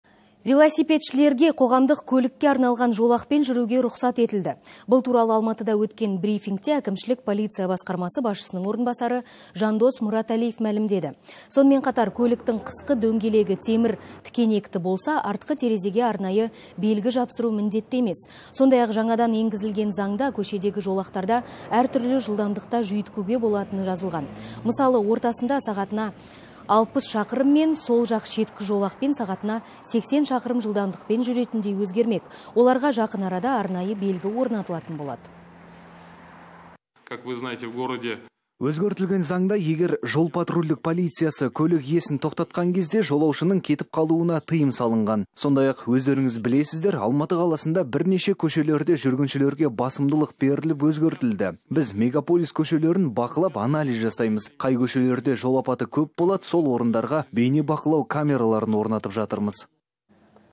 Велосипедшілерге қоғамдық көлікке арналған жолақпен жүруге рұқсат етілді (17.11.17)
Велосипедшілерге қоғамдық көлікке арналған жолақпен жүруге рұқсат етілді. Бұл туралы Алматыда өткен брифингте әкімшілік полиция басқармасы басшысының орынбасары Жандос Мұратәлиев мәлімдеді. Сонымен қатар, көліктің қысқы дөңгелегі темір тікенекті болса, артқы терезеге арнайы белгі жапсыру міндетті емес.